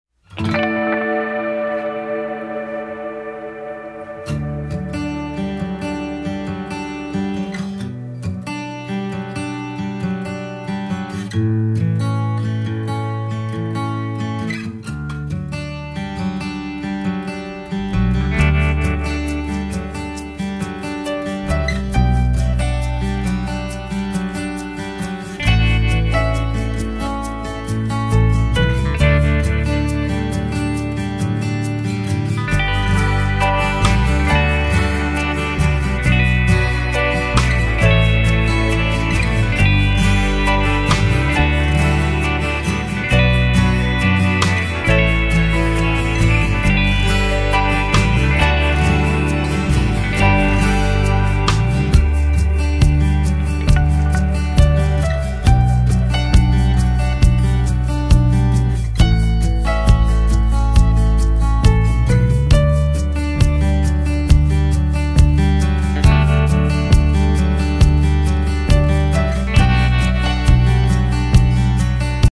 Sylhet Tea garden
Video is taken from near the khashia  village ,Sylhet, Bangladesh .